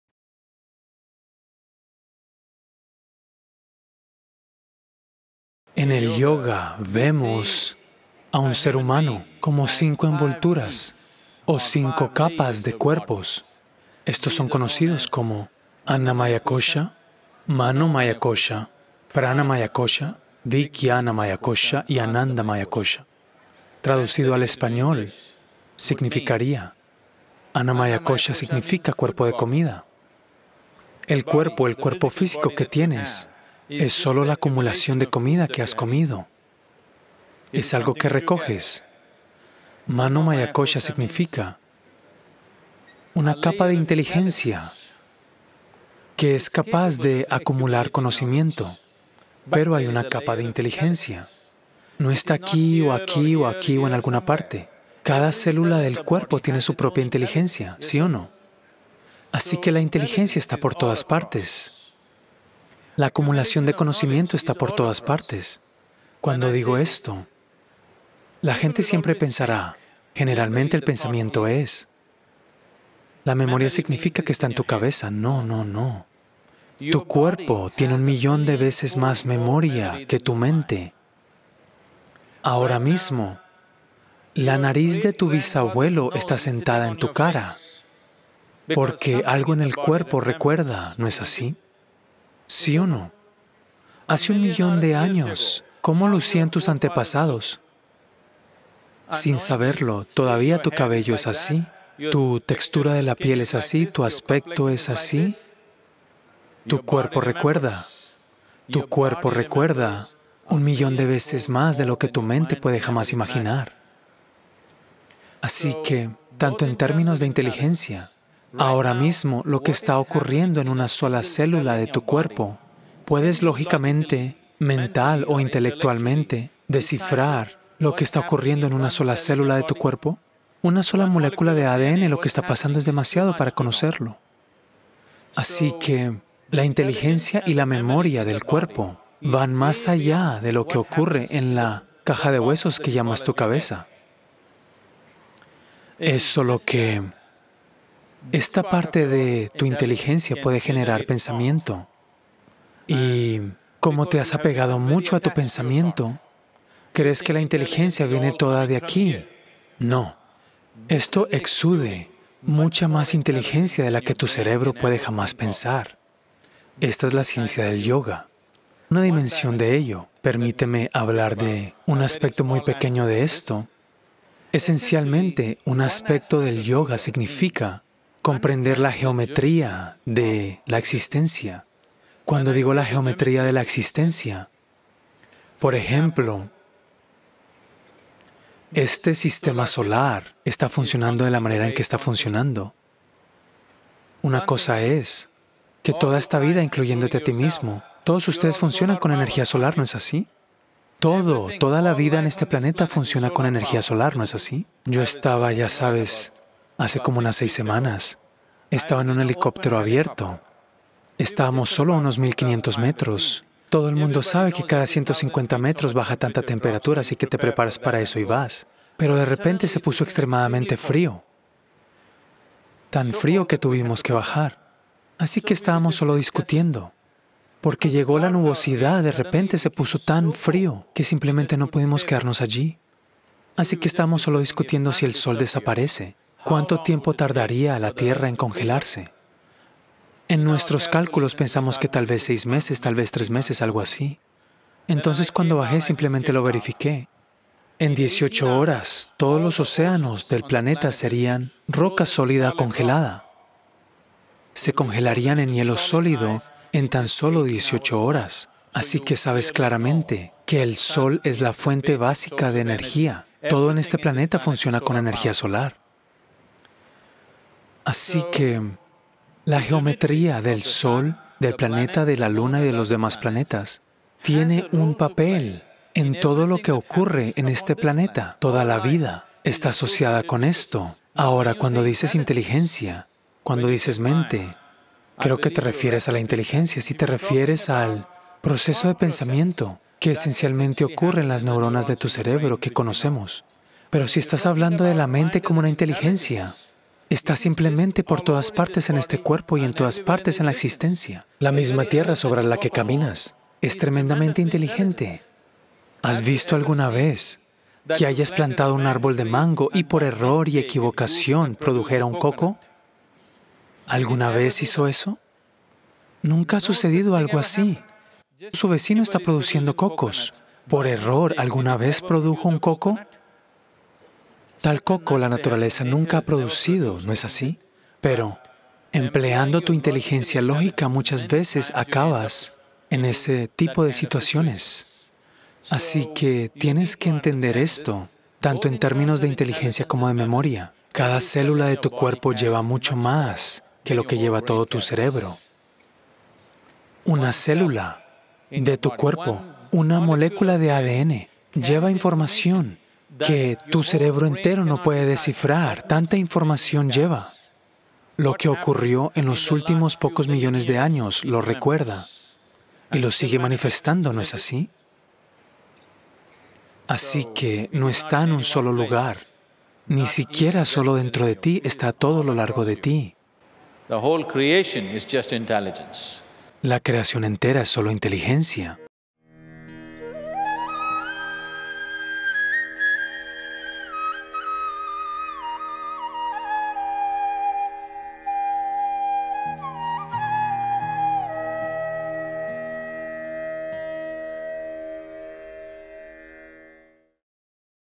¿En qué parte del cuerpo está la mente? | Sadhguru
Sadhguru responde a una pregunta sobre dónde se encuentra la mente en el cuerpo y explica que no es solo el cerebro el responsable de la inteligencia: cada célula del cuerpo tiene una inteligencia fenomenal. Explica que la inteligencia está en todas partes de la existencia.